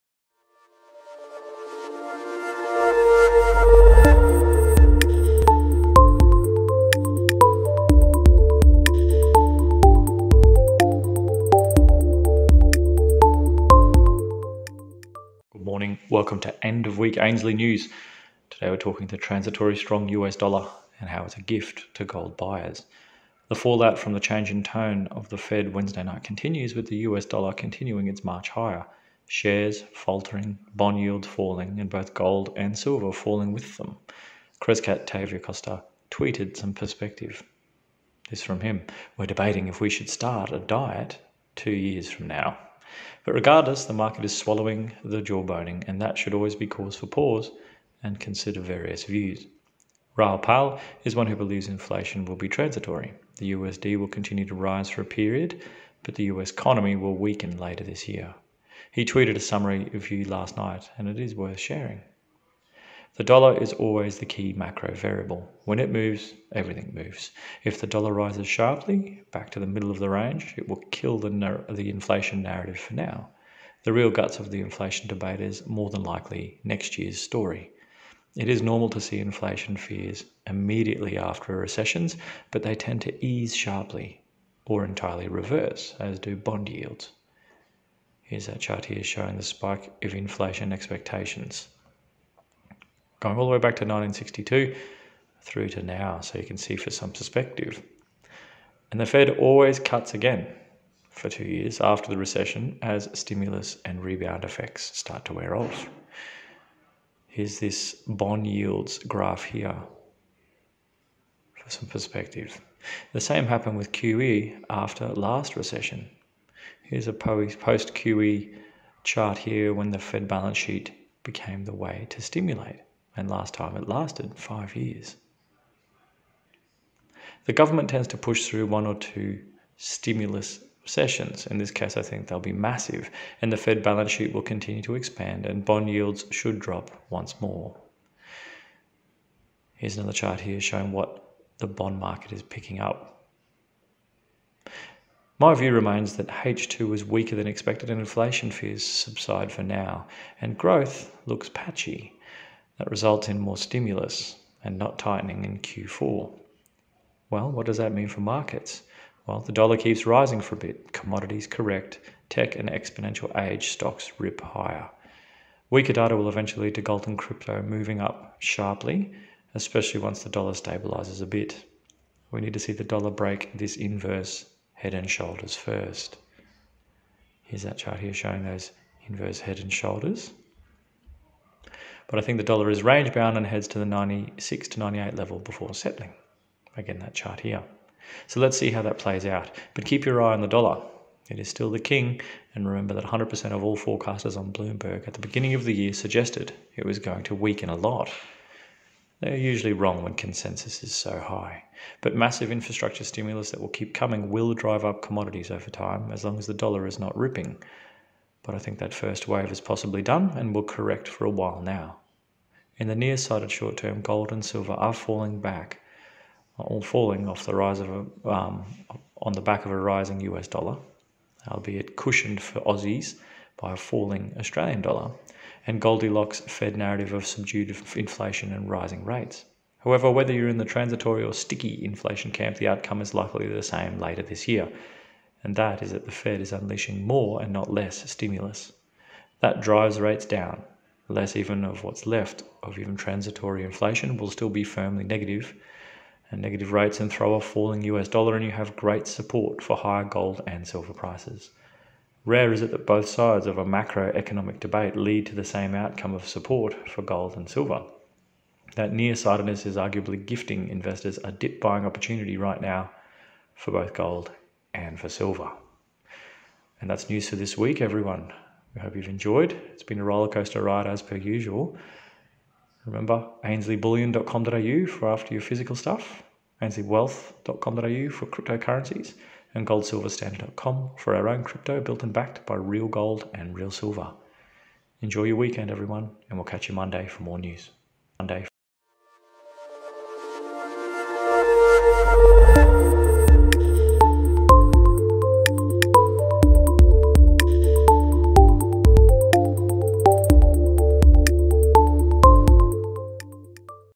Transitory Strong USD Gift to Gold Buyers
The fallout from the change in tone of the Fed Wednesday night continues with the USD continuing its march higher, shares faltering, bond yields falling and both gold and silver falling with them.  We present the other side to the non transitory inflation camp and why it too is good for gold.